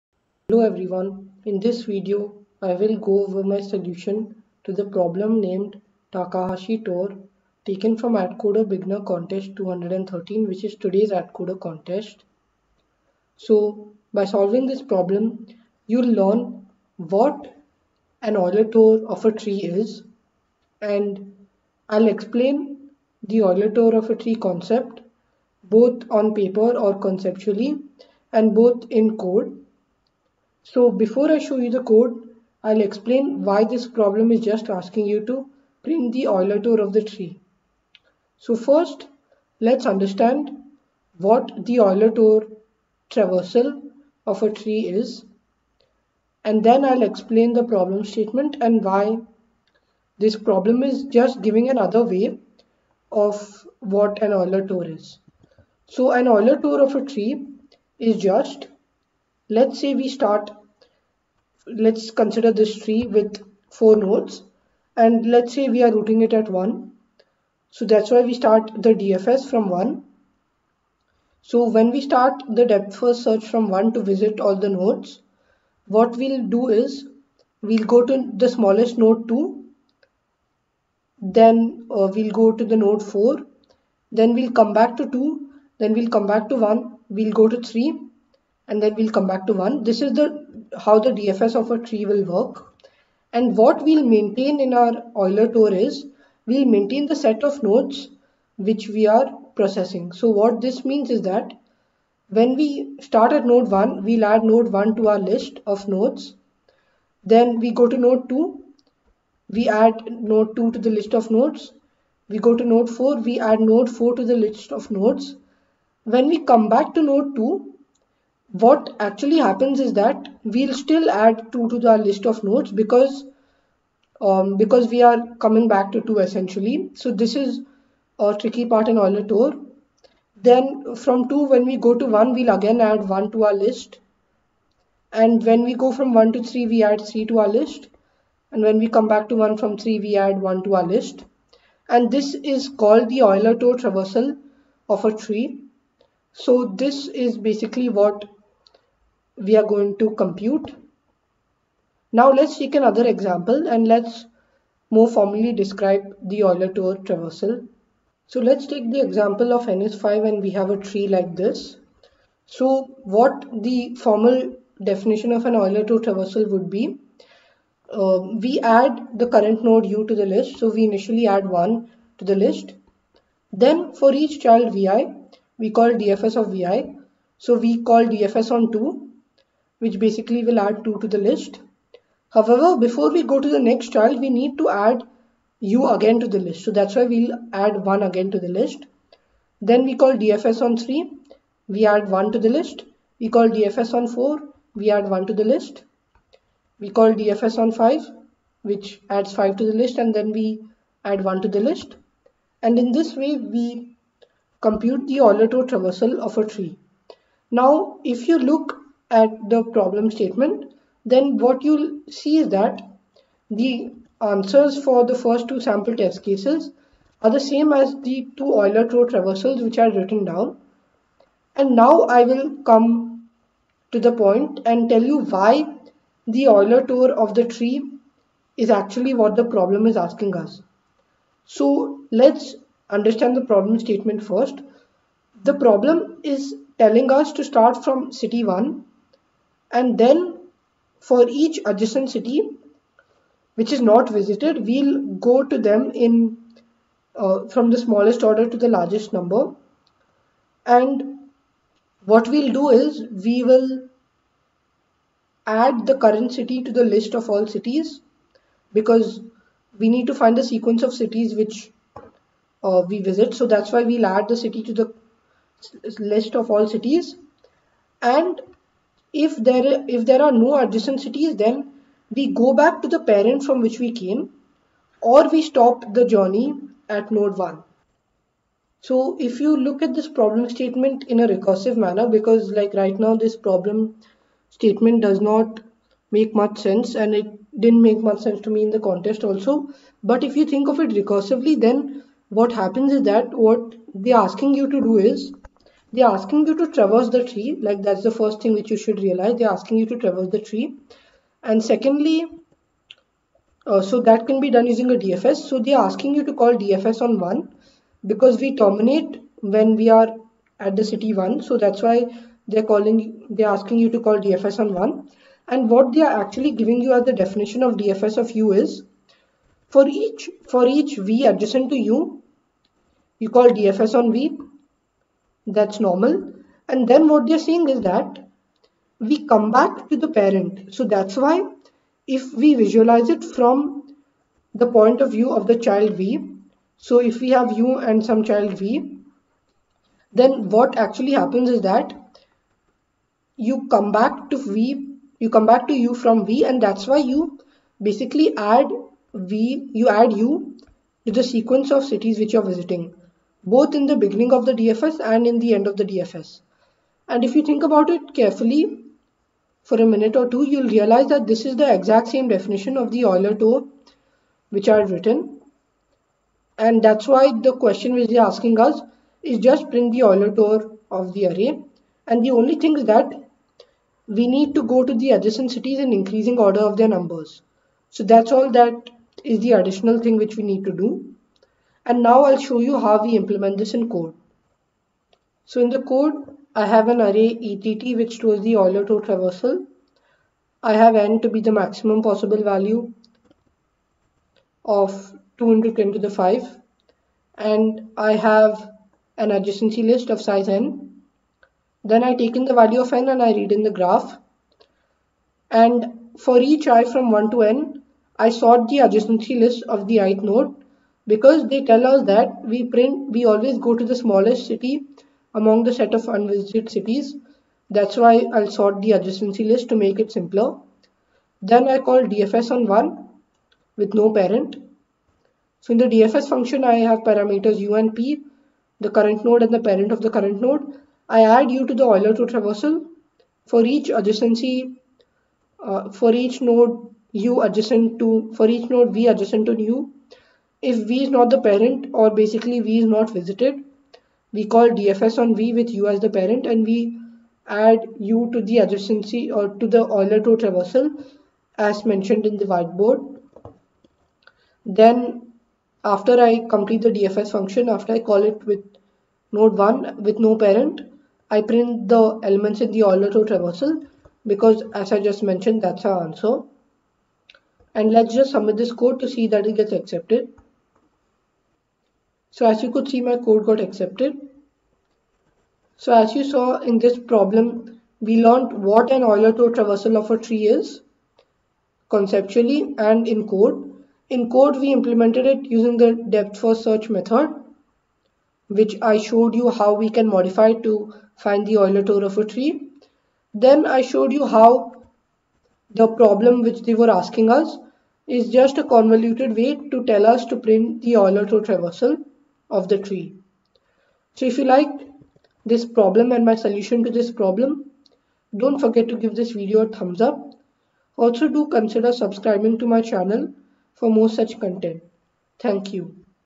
Atcoder Beginner Contest: 213 D: Takahashi Tour - Euler Tour Traversal in a Tree; DFS
Atcoder Beginner Contest: 213 D: Takahashi Tour - Euler Tour Traversal in a Tree; Depth First Search

What will you learn from this video editorial?:
In this video, you will learn what the Euler tour Traversal of a tree is and how we can find it using a depth first search.

Then you will understand why this problem is just asking you do find the Euler Tour Traversal of this tree.

Timestamps:
0:00 – intro
0:15 –Learning target - Euler Tour Traversal of a tree
0:38 – Explanation of Euler Tour Traversal of a tree
2:35 – More formal definition of Euler Tour Traversal
3:50 – Problem statement explanation and solution sketch
6:24 – the code
7:55 – lessons learnt (using DFS to do the Euler Tour)

Hope you are able to solve many of the problems in the above playlist.